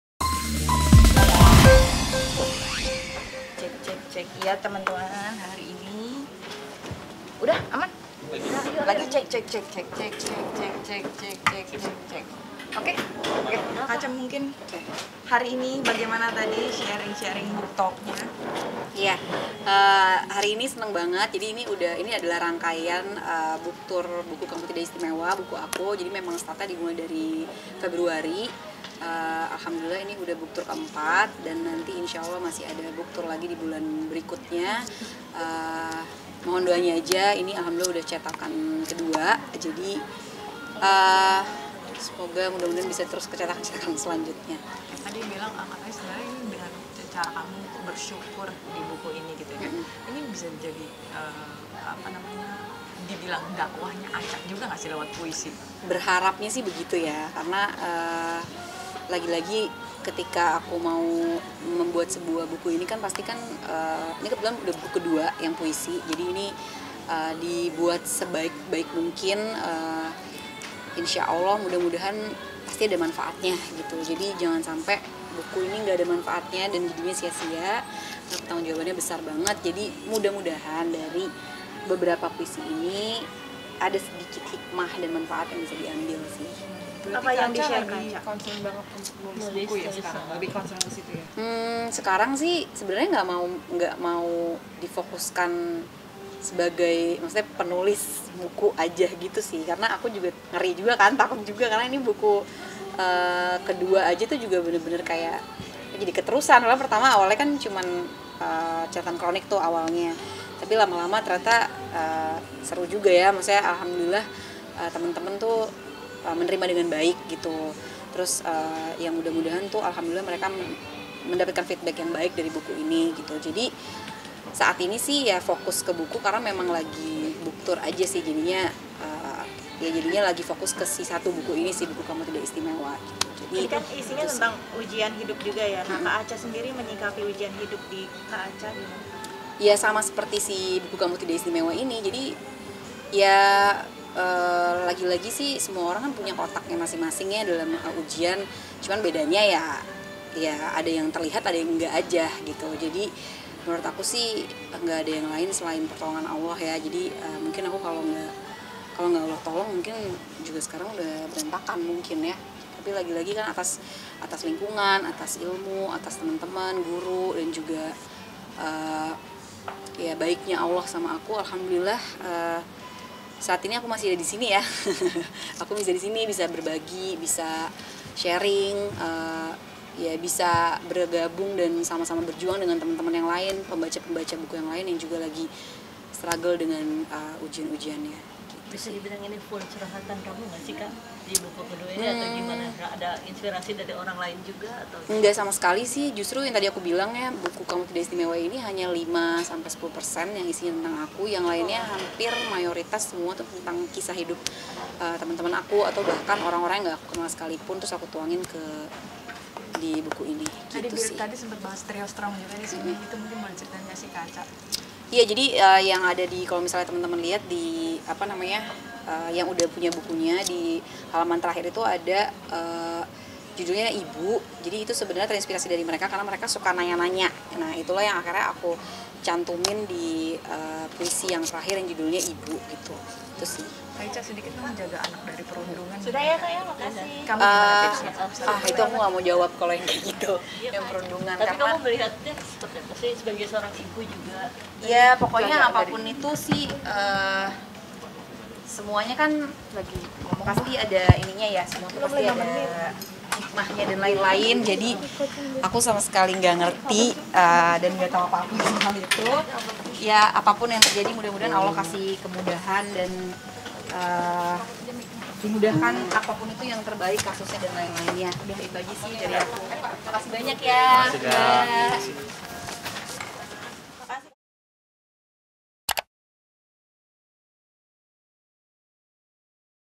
Fokus Jadi Penulis, Natasha Rizky Berharap Bukunya Bermanfaat Untuk Banyak Orang
Natasha Rizky baru saja merilis buku terbarunya, Natasha Rizky berharap bukunya bisa bermanfaat untuk banyak orang.